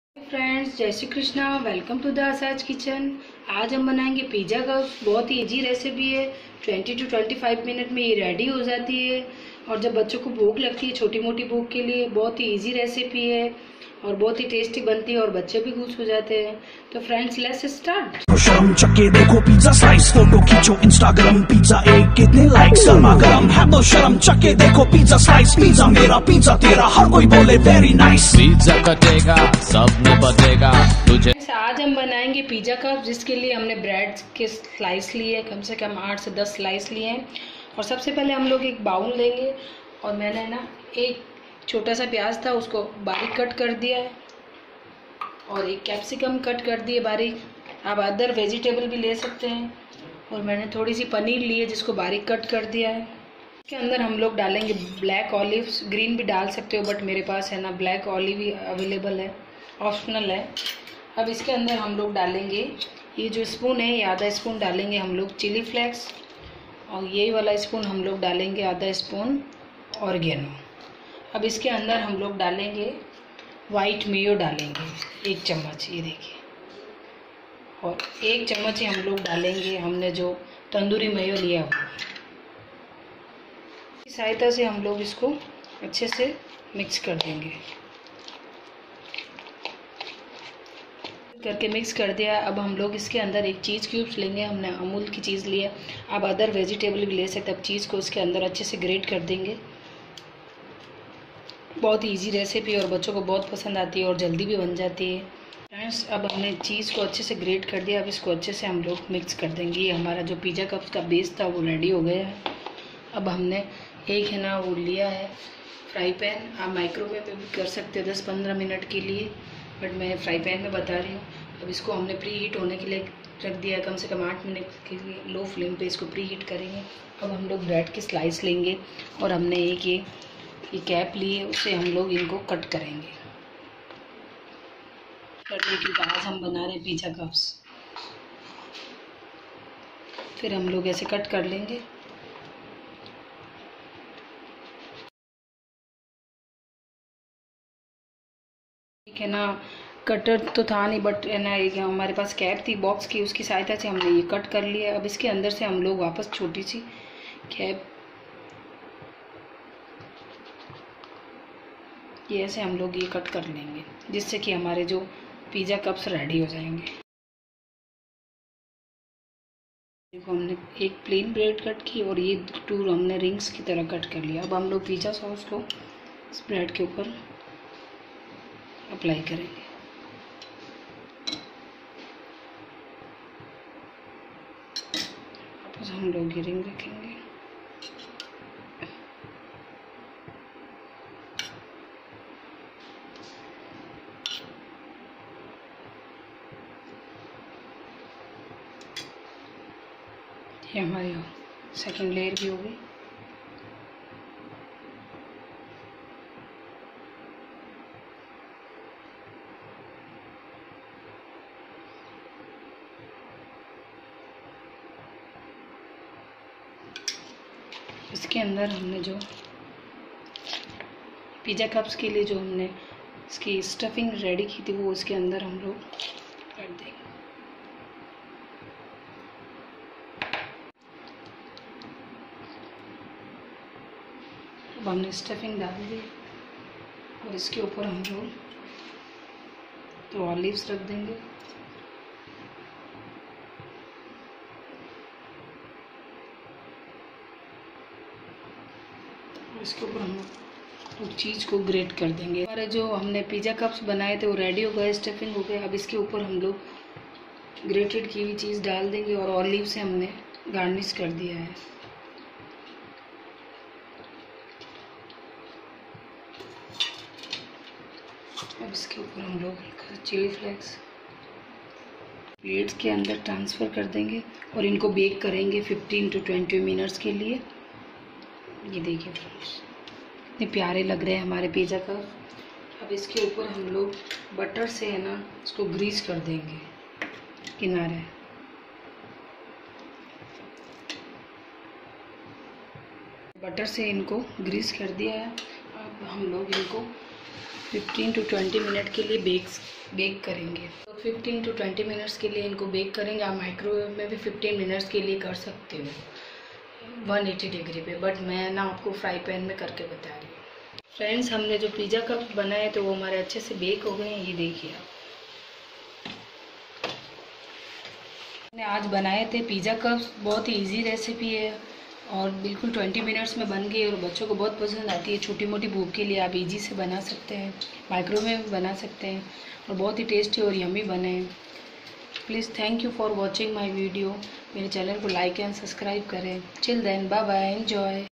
Pizza 🍕 cups recipe / homemade recipe.. pizzalove❤❤ #🍕pizzaaa
Hiiii Frnds.
It's a homemade and very easy recipe..
Frnds PL try these pizza cups...
it's taste is Osum and same like simple pizza...
pizza🍕love
ingredients -

Bread🍞
cheese
olives
capsicum
onion
panner
pizza sauce
and Frnds last but not least
 ketchup (optional) ...
Chilli flakes
organo
salt to taste
Tandoori Mayonise
White Mayonise